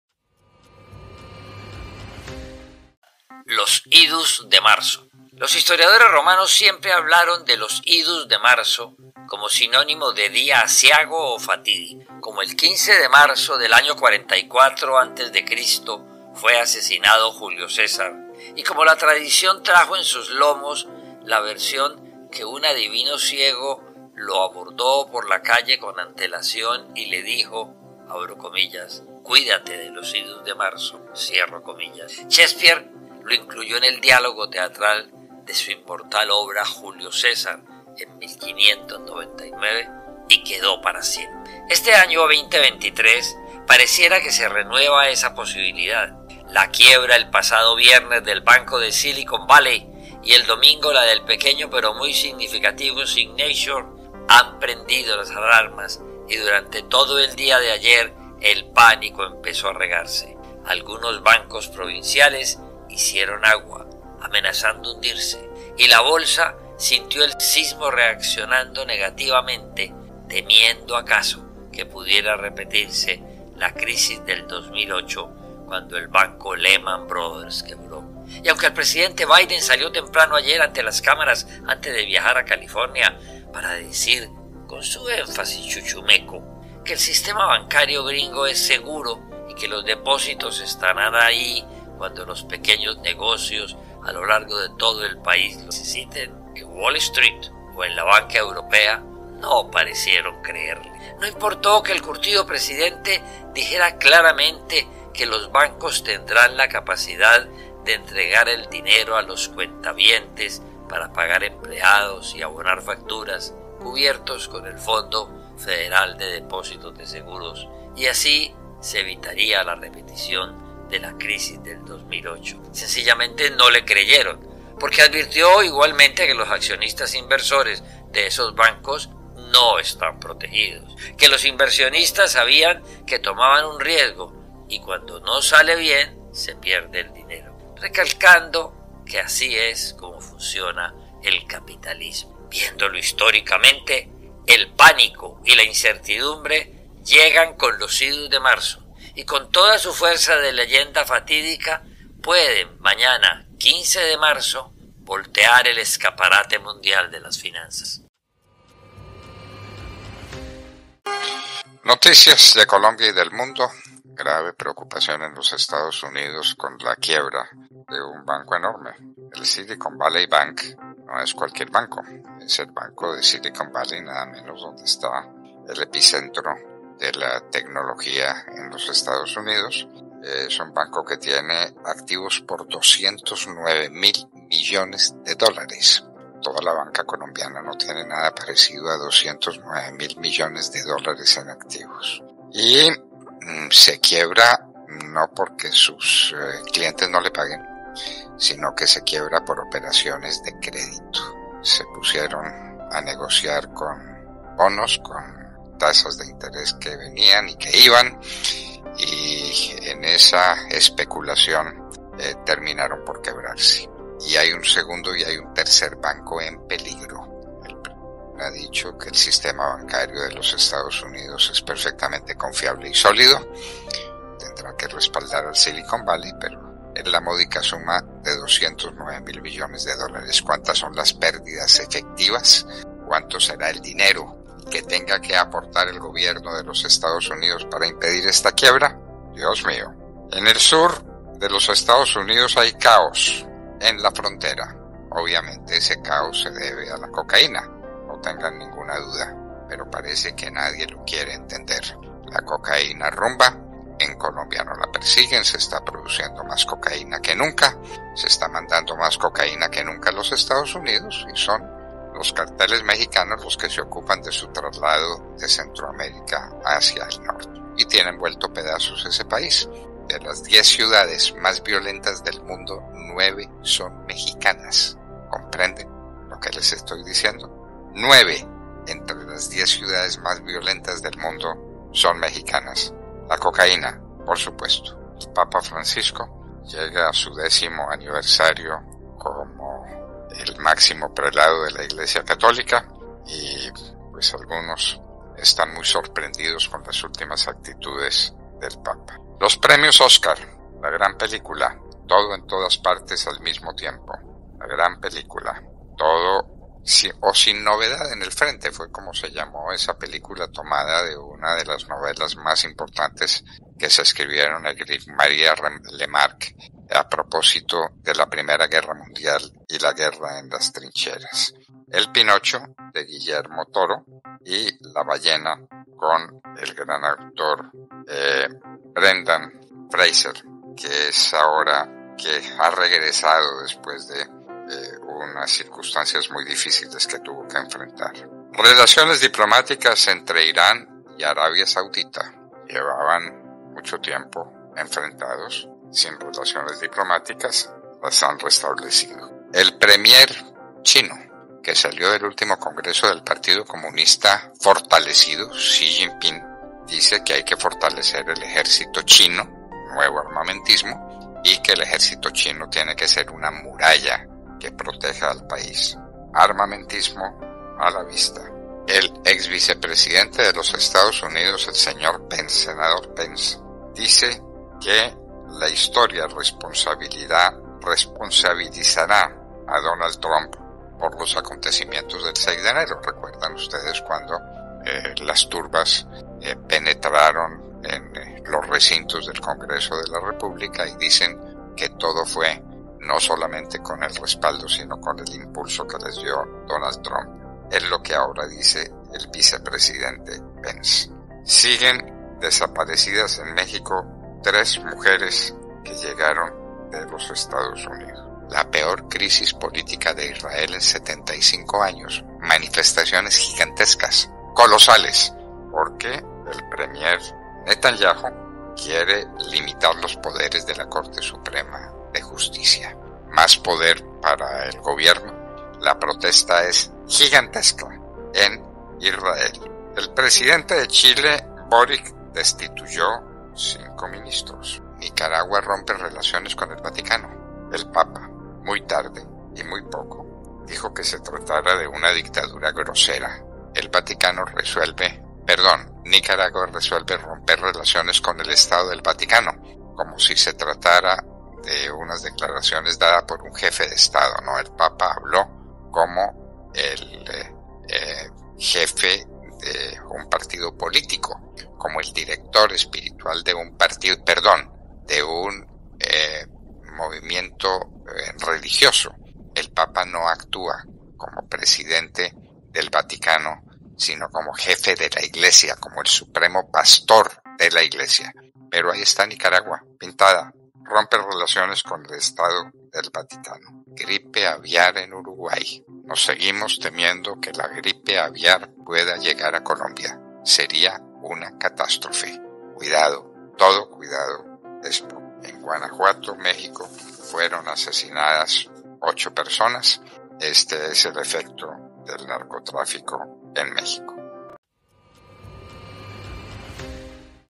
LOS IDUS DE MARZO || Crónica Gardeazábal ➕ noticias del
Parecería que los miticos y fatídicos  IDUS DE MARZO, elevados como episodidos inatajables  en la historia universal por Julio César y Shakespeare, están a punto de repetirse con la crisis bancaria que empanica al mundo.
cronica Gardeazábal
Titulares del Mundo con Fernando Londoño